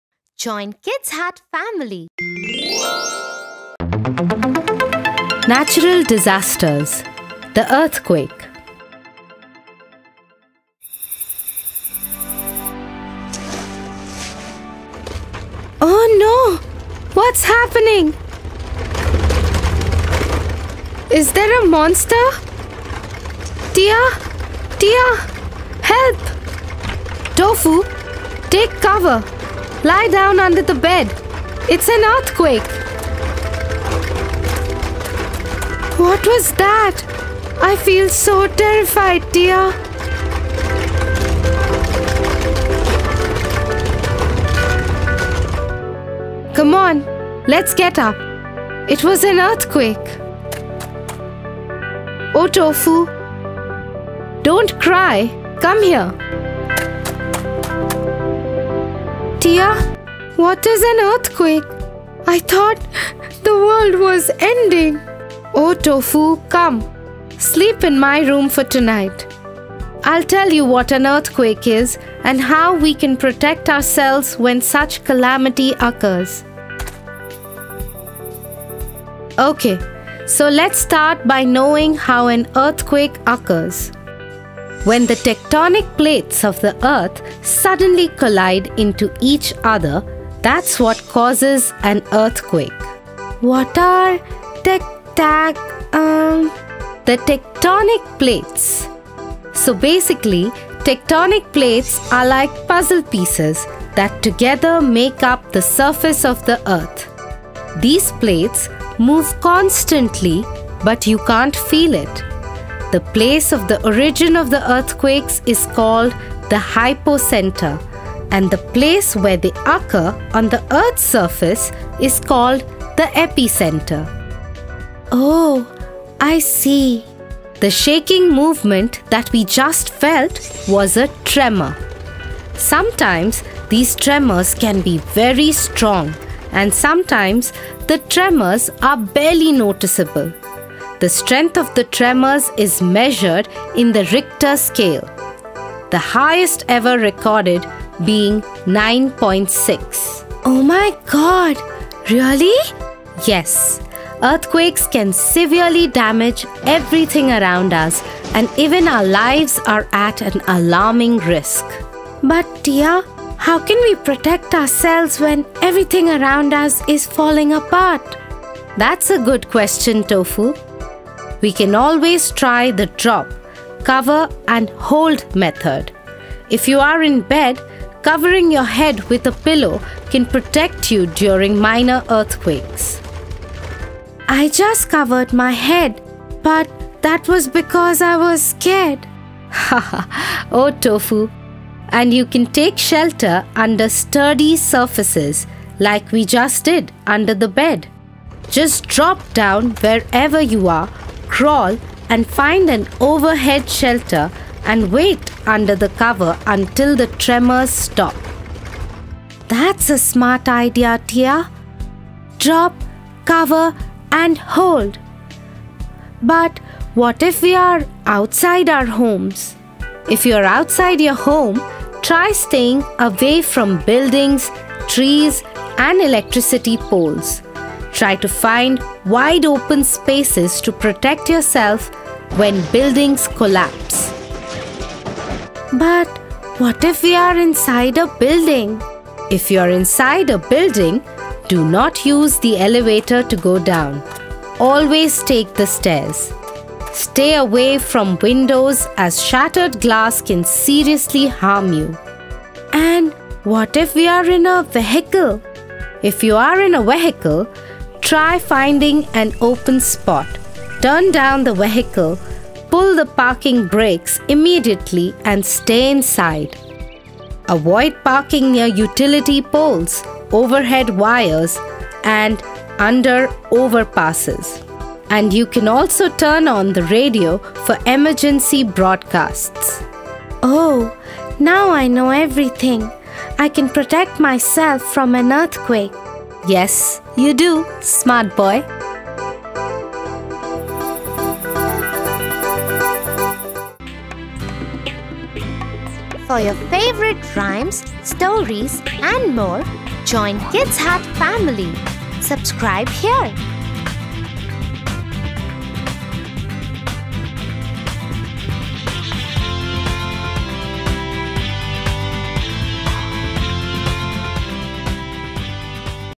NATURAL DISASTERS : THE EARTHQUAKE | Stories For Kids In English | TIA & TOFU Lessons For Kids
Kids Hut family presents "NATURAL DISASTERS : THE EARTHQUAKE" by KIDS HUT.

✿ Voice Credit: Tulsi Kumar
✿ Story Credit: Sumra Khan